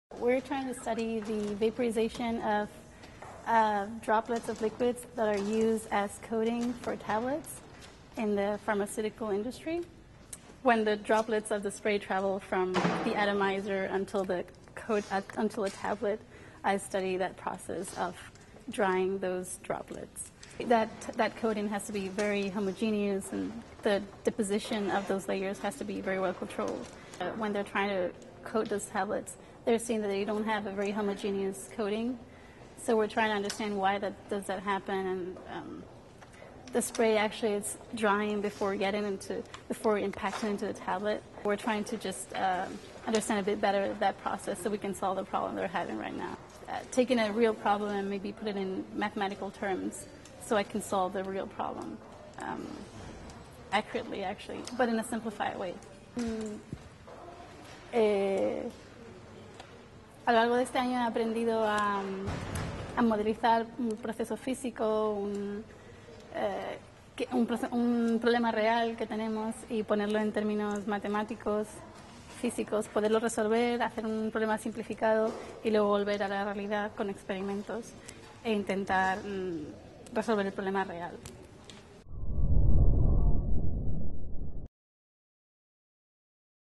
Silvana Sartori on making better pill coatings
Silvana Sartori is a mechanical engineering graduate student at UC San Diego. As she hung her poster at Research Expo 2010, she described her work and her expertise -- in both English and Spanish. Keep up with the Jacobs School of Engineering on our blog and on Twitter